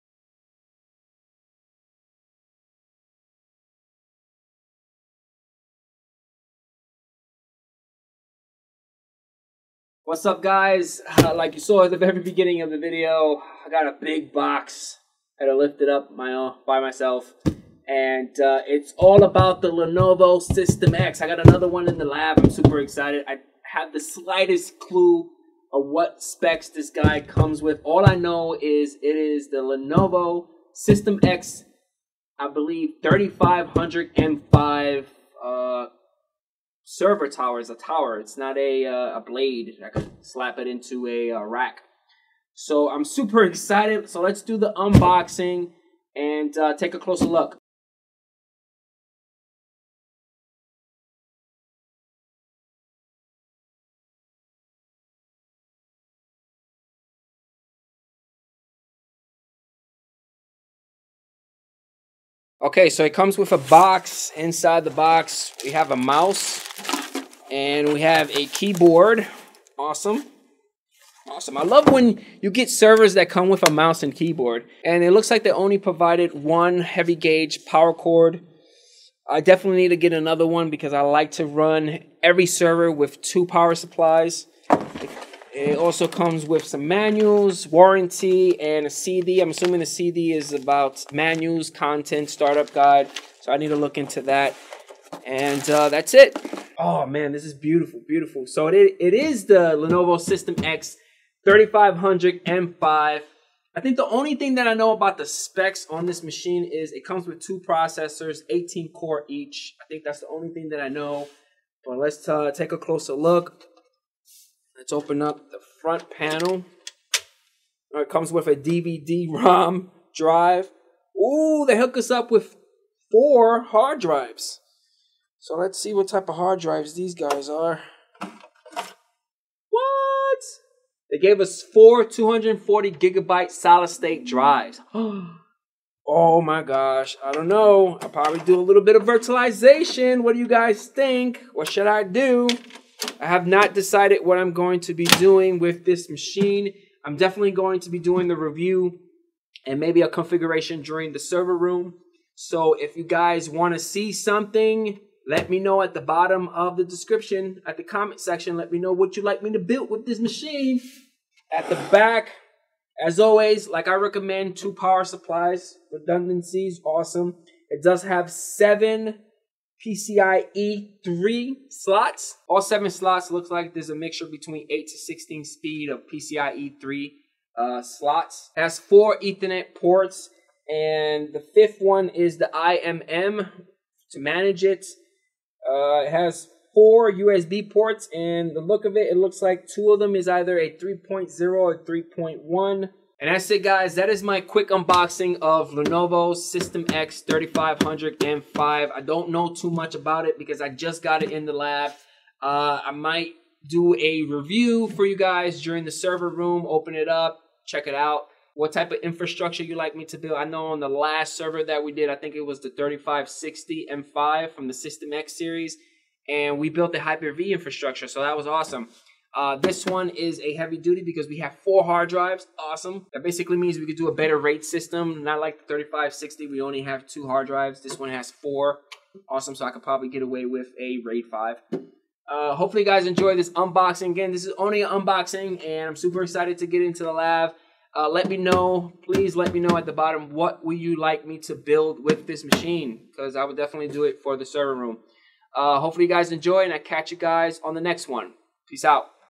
Lenovo System x3500 M5 Server Unboxing!
This week we have the Lenovo System x3500 M5 server in the lab. What should we do with this server? Making some plans on using it for our TheServerRoom show. Any suggestions?